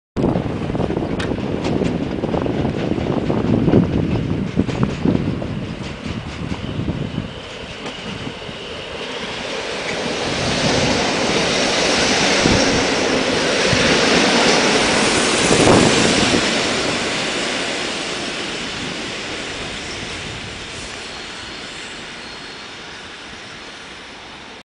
A suburban commuter train Warka to Warszawa Wschodnia arrives at Warszawa Zachodnia (2012)
Osobówka z Warki Kolei Mazowieckich wtacza się na dworzec Warszawa Zachodnia, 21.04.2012 r. EN57-1913. Stary skład Koleje Mazowieckie udostępniły i tak to wygląda właśnie